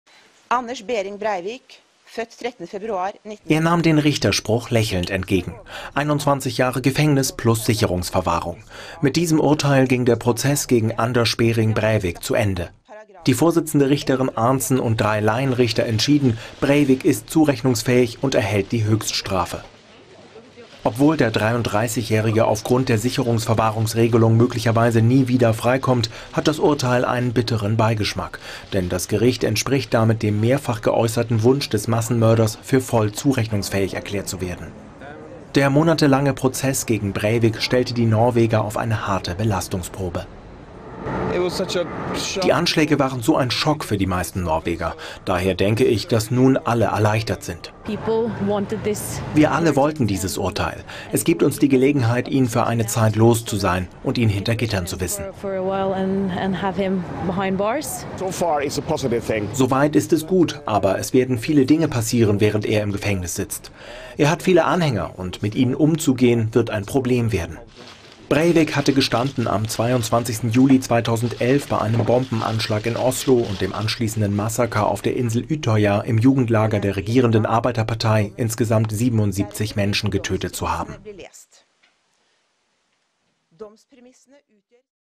Breivik-Schuldspruch: "Jetzt sind alle erleichtert" | DER SPIEGEL
Massenmörder Anders Breivik hält sich für geistig gesund -und genauso sehen es die Richter: Für den Mord an 77 Menschen verurteilten sie ihn zu 21 Jahren Haft plus Sicherungsverwahrung. Der monatelange Prozess gegen Breivik war für viele Norweger eine Belastungsprobe.